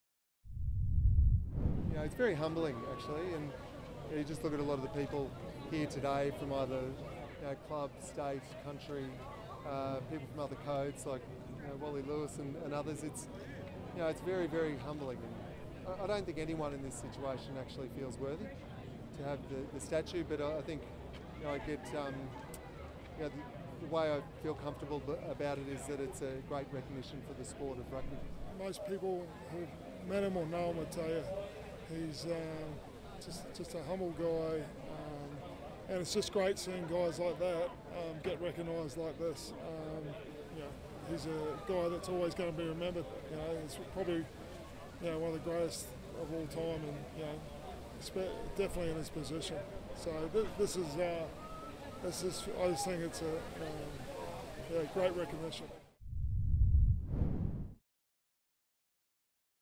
John Eales honoured with statue
The Queensland Rugby Union has honoured Queensland Rugby great John Eales with a historic statue at Suncorp Stadium - the first Rugby sculpture to be erected at the venue.